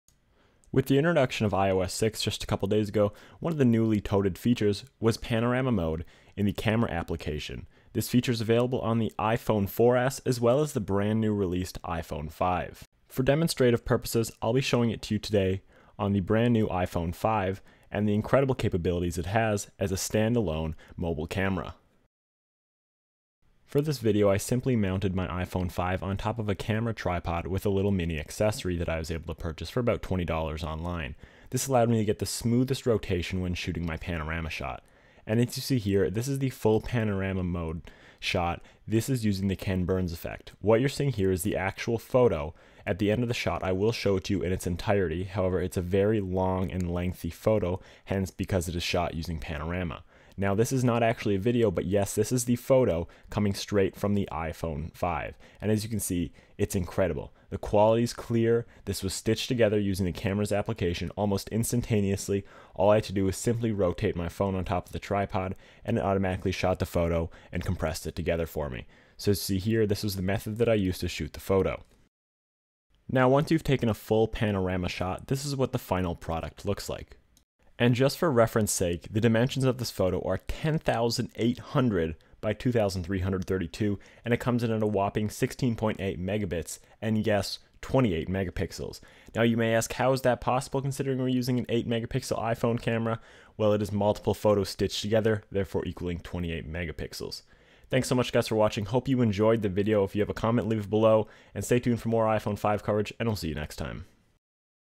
iPhone 5 Panorama Mode
Panorama Mode is a shooting feature available on the iPhone 4S and the newly released iPhone 5 running iOS 6. The results are actually quite astonishing this time around. The pictures I have taken look amazing on the phone and any computer. Take a look for yourself and let me know how you think they look, and what you think about panorama mode.

This is questionably one of the best videos I have shot on this channel in my opinion, let me know what you think?